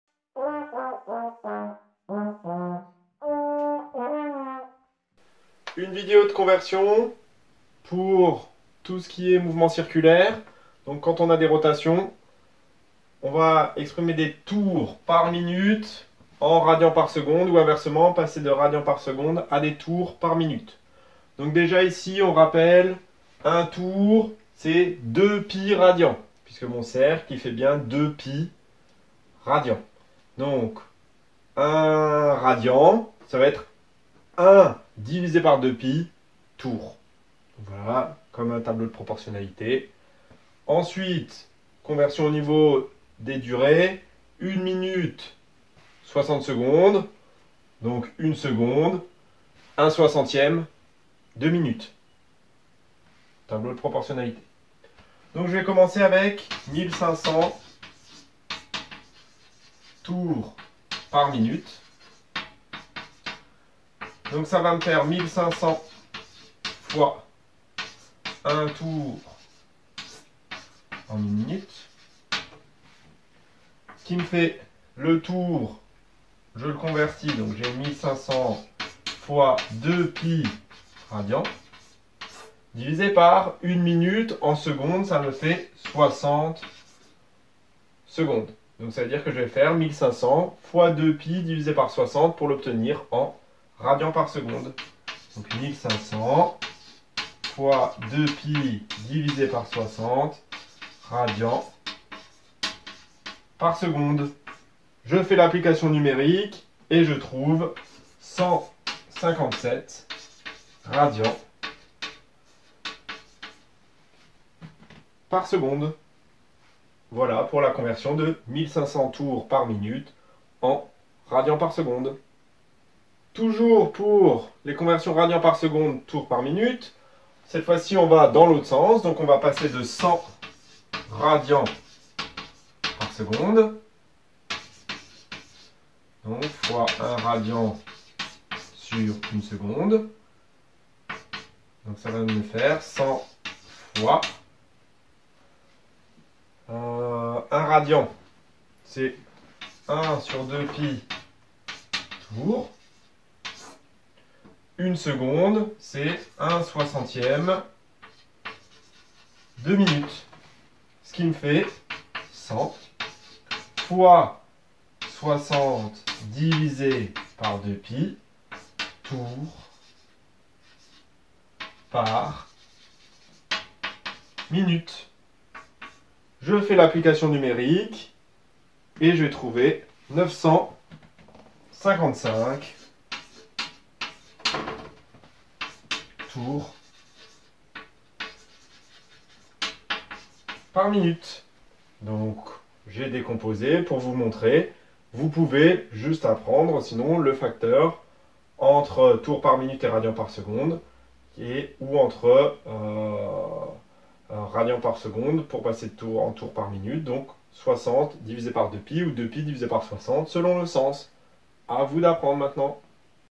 Convertir des vitesses de rotation - Première STI2D - Physique-Chimie - 1STIDD
Chapitre : Cinématique - Mécanique -
Convertir des vitesses de rotation - radian par seconde (rad/s) en tour par minute (tr/min)
2nde - Première 1ère STIDD - spécialité physique-chimie et mathématiques
Lycée Couffignal Strasbourg - 1STI2D - De bonnes bases pour le BTS